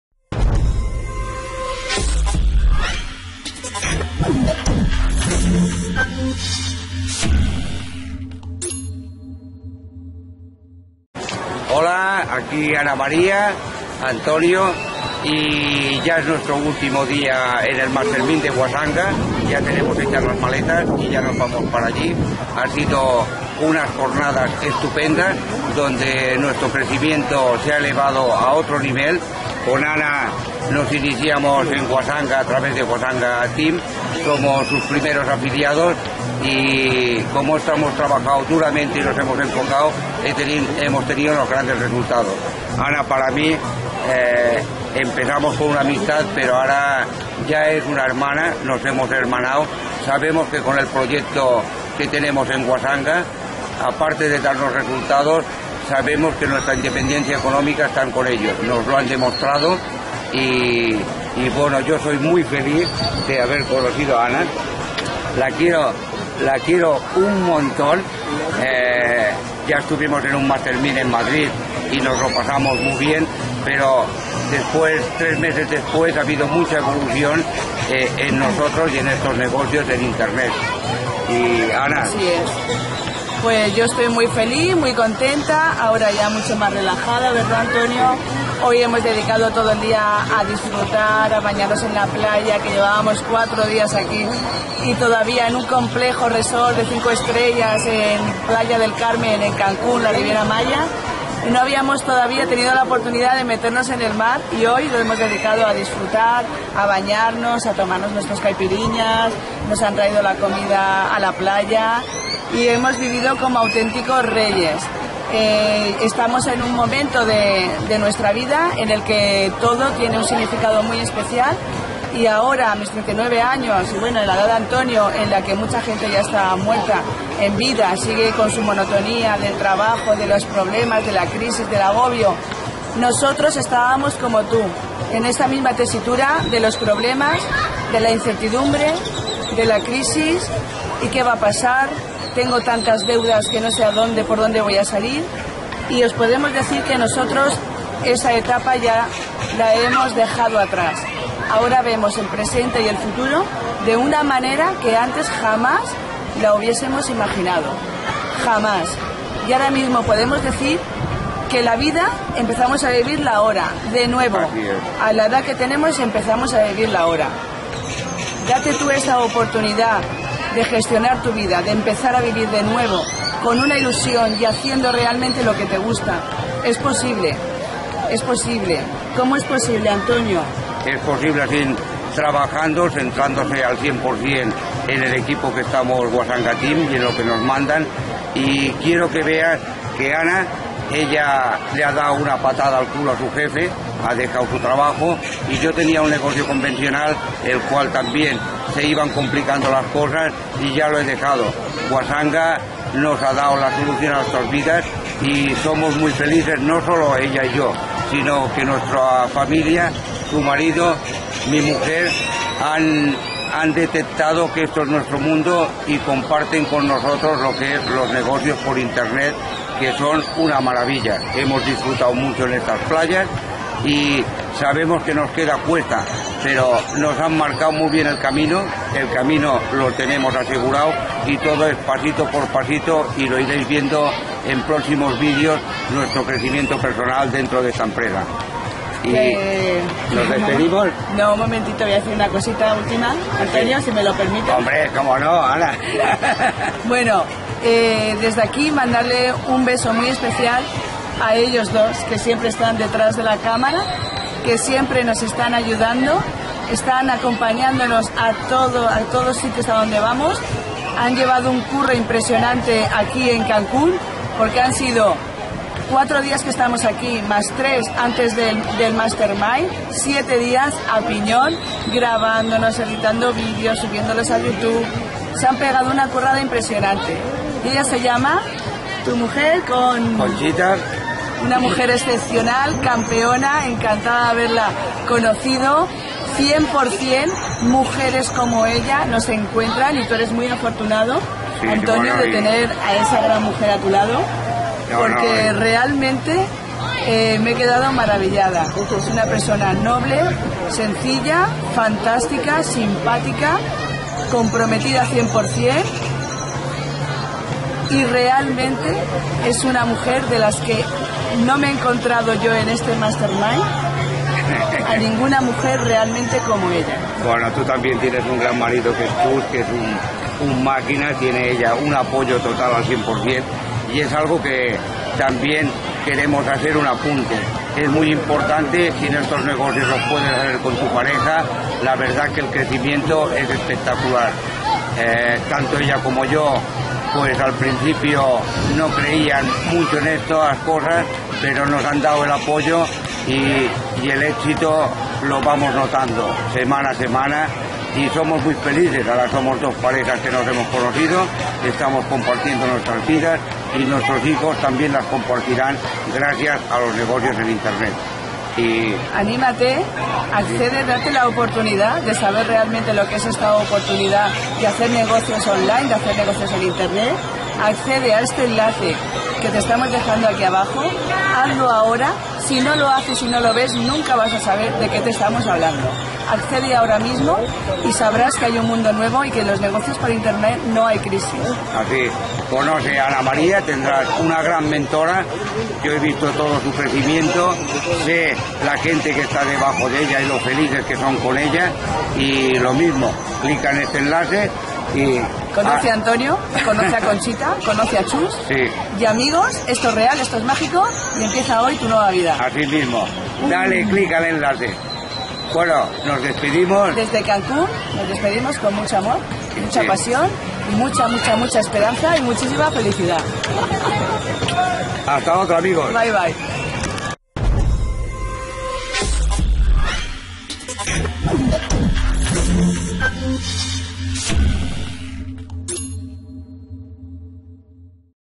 En Cancun con Ana fruto de nuestra amistad en Wasanga.
Wasanga me ha dado mucho y una cosa muy especial conocer a Ana.
Empezamos en el mismo equipo y cosechamos los mismos frutos, somos gente humilde con resultados lejos de ser gurus y vendedores de crecepelo.
Si nosotros lo hemos conseguido en muy poco tiempo, Porque tu no?